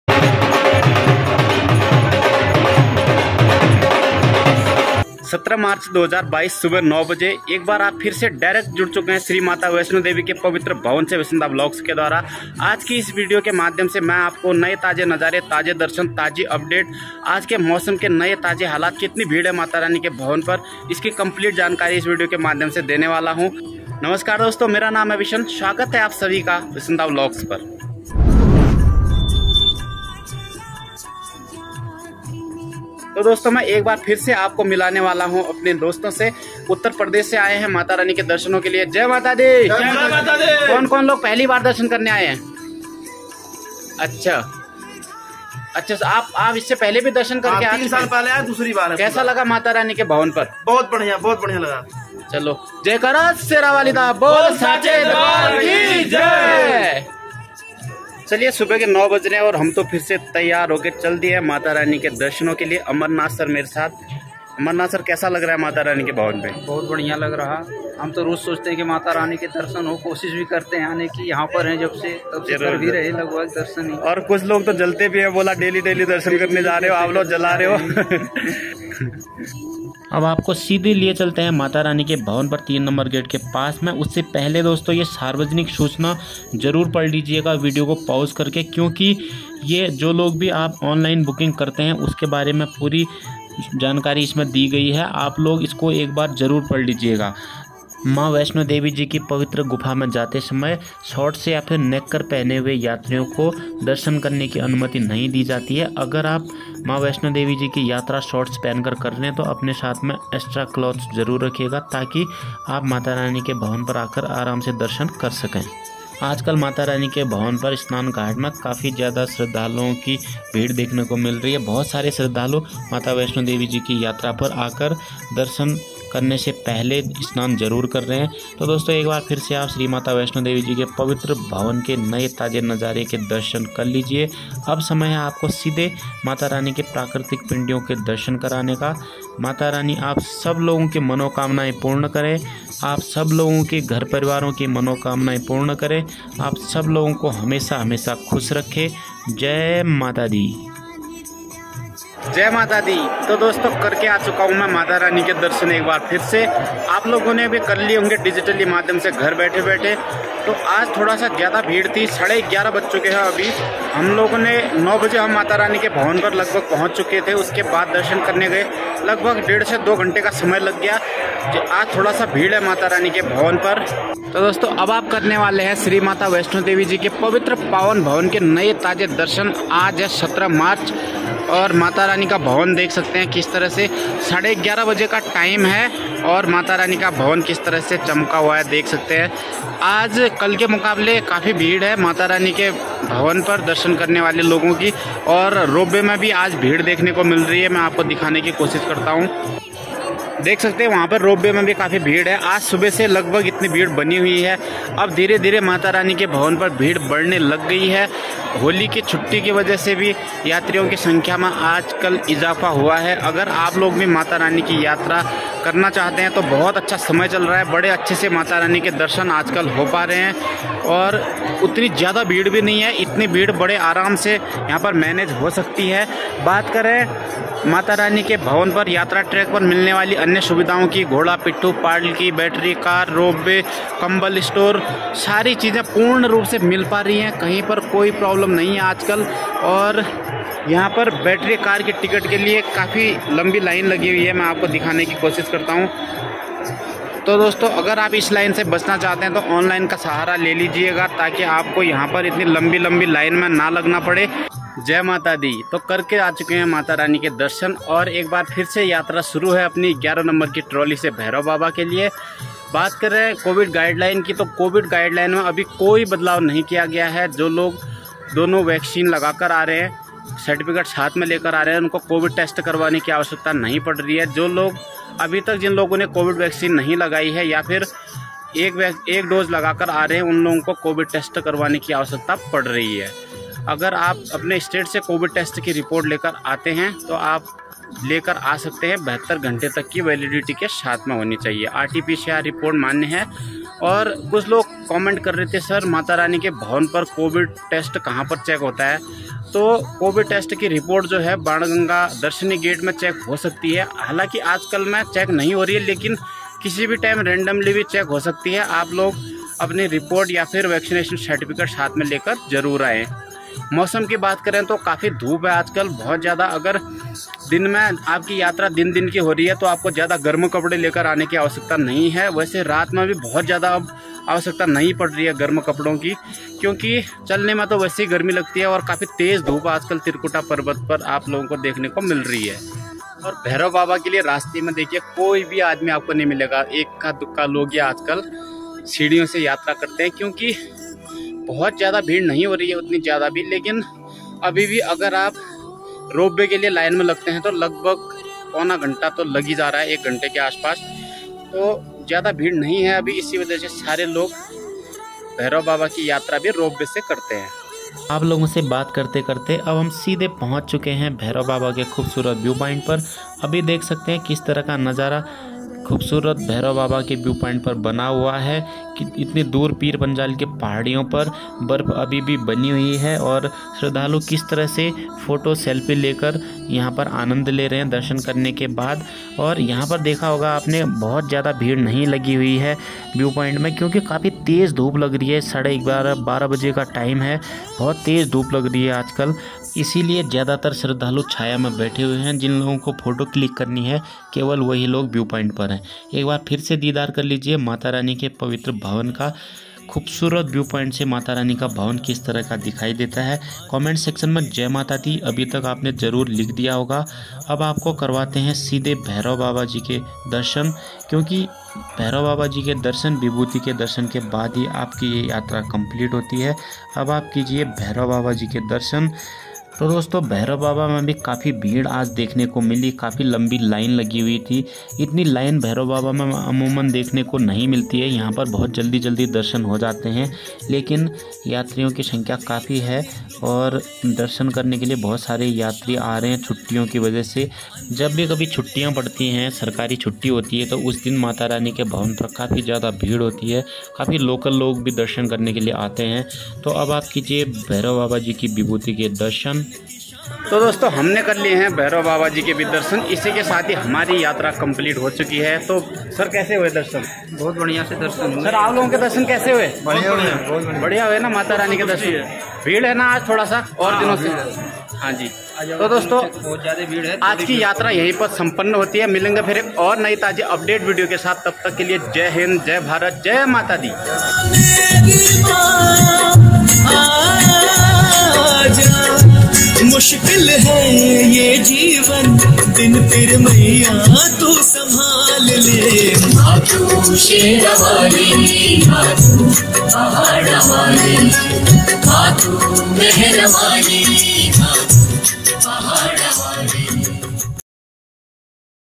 वैष्णो देवी आज के ताजा नजारे।भक्तो की भीड़।मौसम। ताजा दर्शन। Vaishnodeviyatra2022
ardhkuwari ki kuch khas baten.